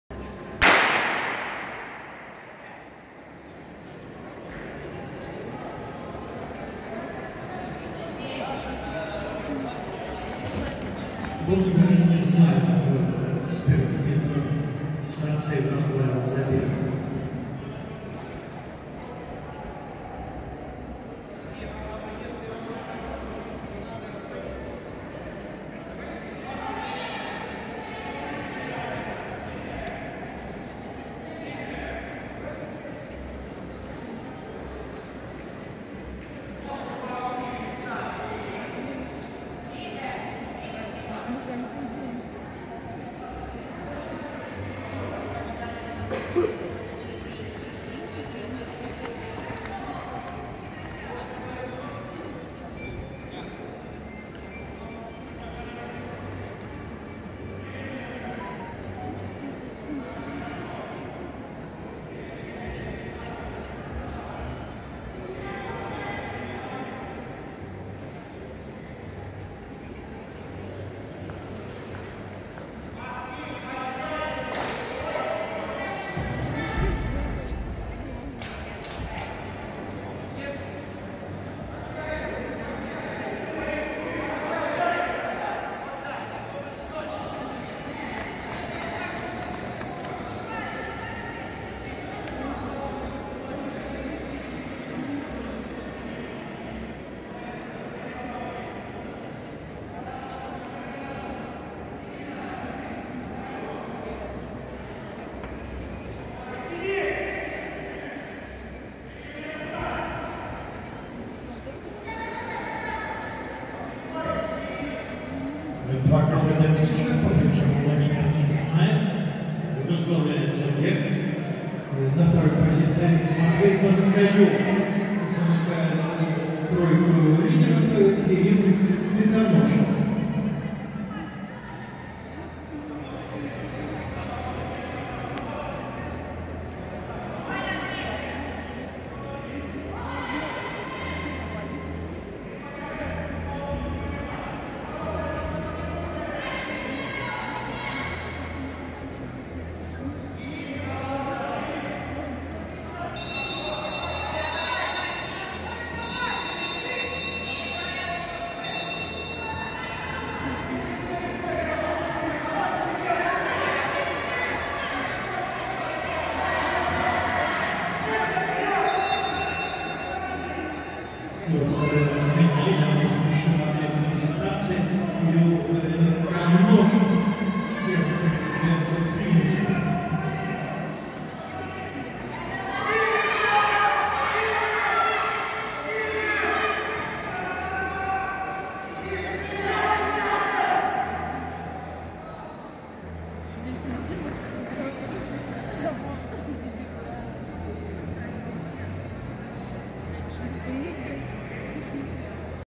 Финальные забеги на 1000 м юношей 2006-2007 г.р. (25.04.2021).
Третий, завершающий, день весеннего первенства ГБУ СШОР «Академия легкой атлетики Санкт-Петербурга». Соревнования среди юношей и девушек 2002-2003, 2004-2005 и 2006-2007 г.р. (Зимний стадион, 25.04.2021).